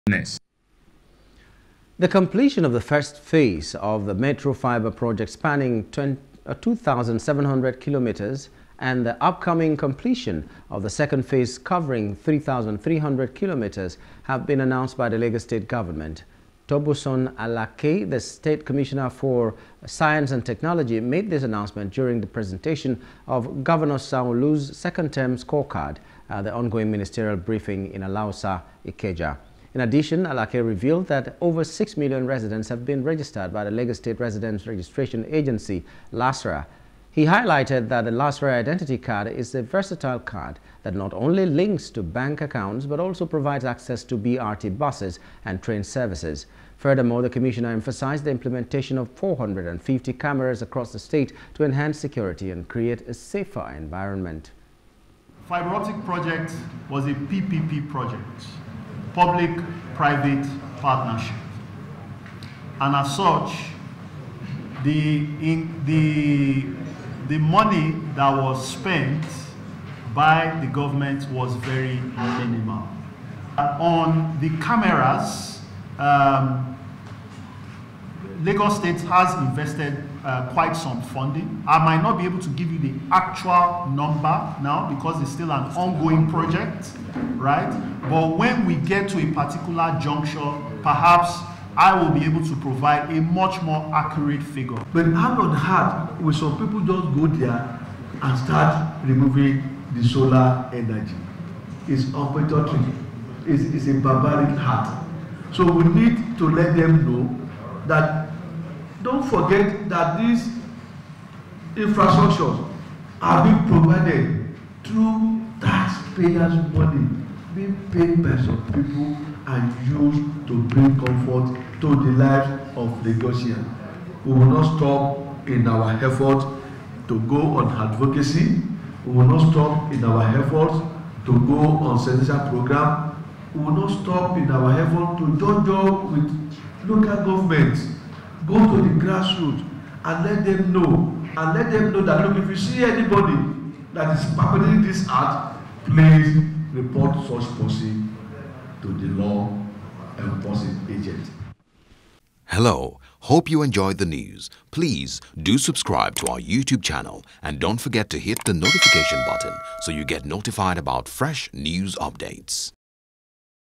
Metro Fibre Project Lagos Govt Completes First Phase, Spanning 2700km
The completion of the first phase of the Metro Fibre Project, spanning 2,700km, and the upcoming completion of the second phase, covering 3,300km have been announced by the Lagos State Government.
Tubosun Alake, the State Commissioner for Science and Technology, made this announcement during the presentation of Governor Sanwo-Olu's second term scorecard at the ongoing Ministerial briefing in Alausa, Ikeja.